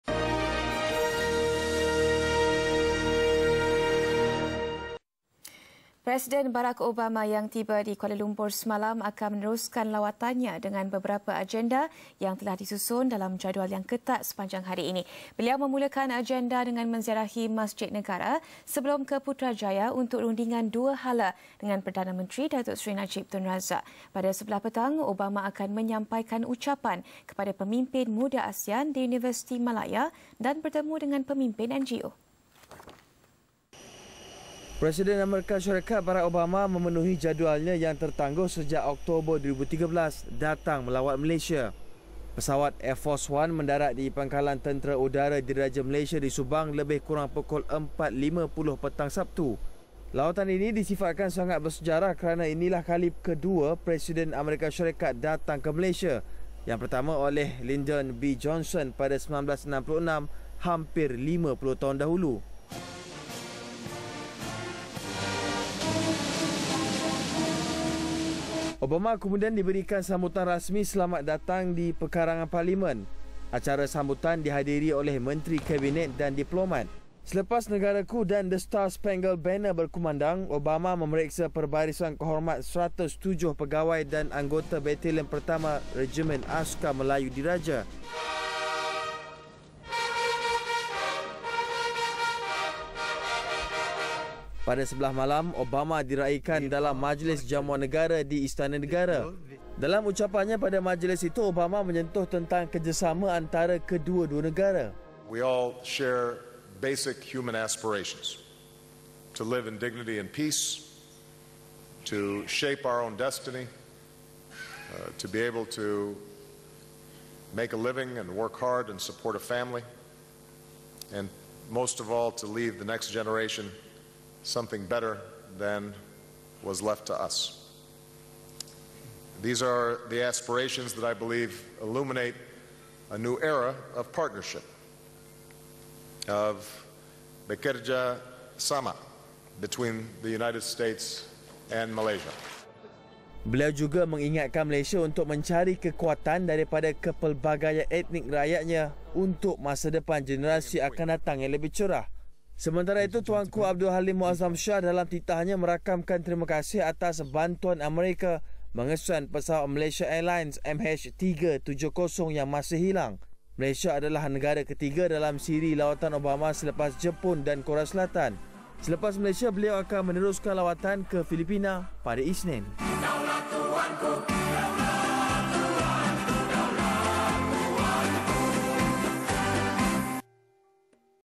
Hari pertama Barack Obama di Malaysia
Presiden Barack Obama yang tiba di Kuala Lumpur semalam akan meneruskan lawatannya dengan beberapa agenda yang telah disusun dalam jadual yang ketat sepanjang hari ini.

Beliau memulakan agenda dengan menziarahi Masjid Negara sebelum ke Putrajaya untuk rundingan dua hala dengan Perdana Menteri, Datuk Seri Najib Tun Razak.

Pada sebelah petang, Obama akan menyampaikan ucapan kepada pemimpin muda asean di Universiti Malaya dan bertemu dengan pemimpin NGO.